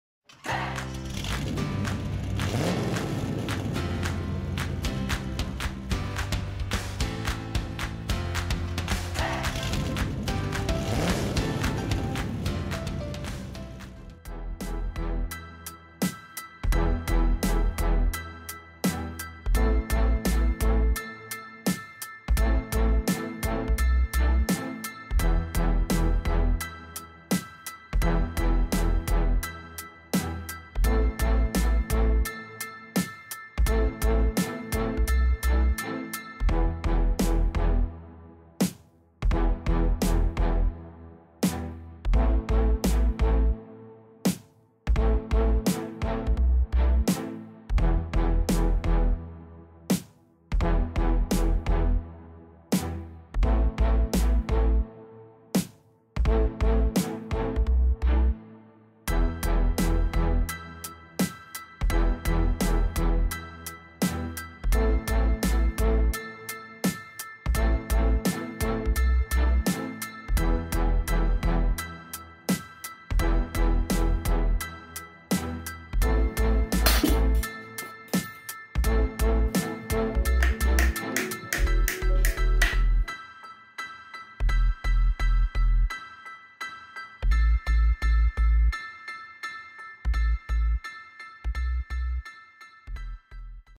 SAFEST AND BEST TRICK FOR REMOVING A STUBBORN DOWEL PIN FROM A LS ENGINE BLOCK TO TRANSMISSION.
If vise grips won't work and you don't want to run the risk of damaging the block or braking off an ear then this is the safest way to remove an Engine to Transmission Steel Alignment Pin also known as a dowel pin. I used a 5/8" impact socket and a 10mm 1/4" drive socket (turned around backwards) to press it out. Be careful it pops like a ball joint when it releases.